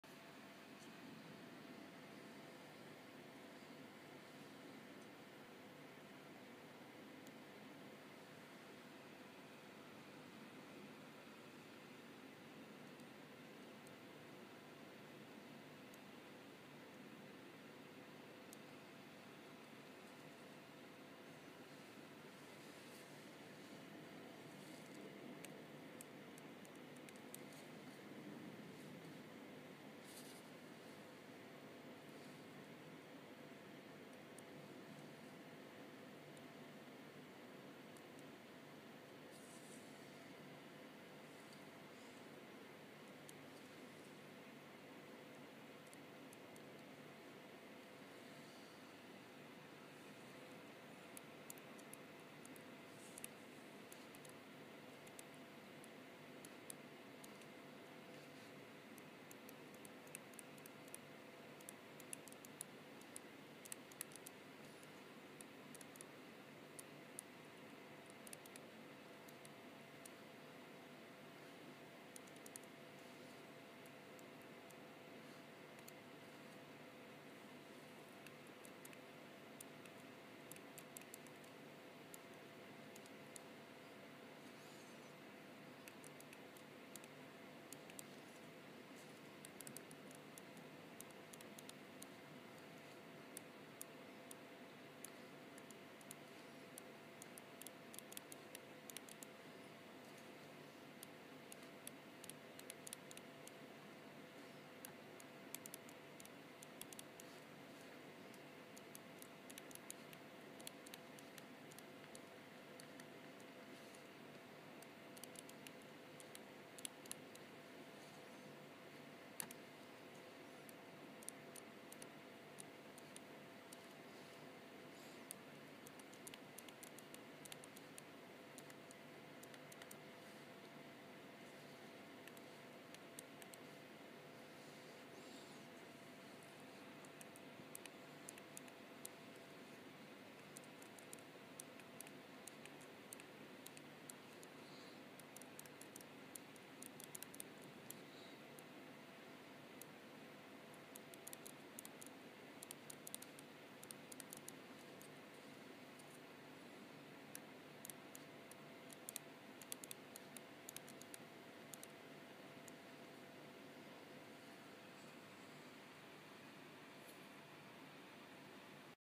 GALAXY Note３でawesomeノートの使用感
GALAXY Note３にawesomeノート(aNote HD)を入れて見ました。Sペンで文字を入力して見ました。
こんな風に意外にも文字認識が早いし正確で良い感じです。
iPadで7notesを使っていましたが、間違いなく、こっちのアプリの方が良いですね！